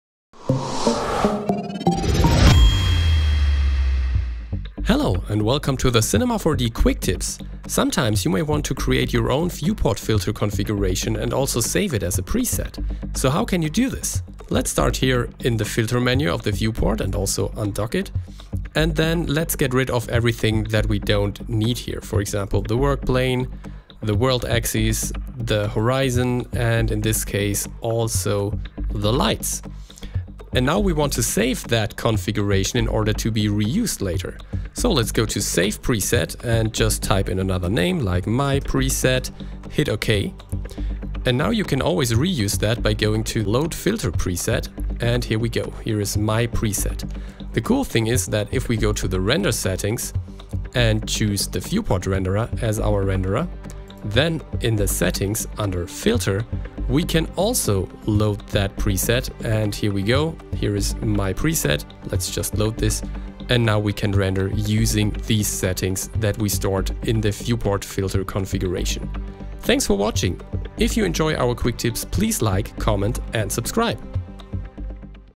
Tip - 299: How to save Viewport Filter configurations as presets in Cinema 4D
In this Quick Tip, Jonas Pilz shows you how to create custom Viewport Filter configurations and save them as presets in order to be re-used. You will also learn how to assign Viewport Filter presets in the render settings of the Viewport Renderer.

Recorded in Cinema 4D S22.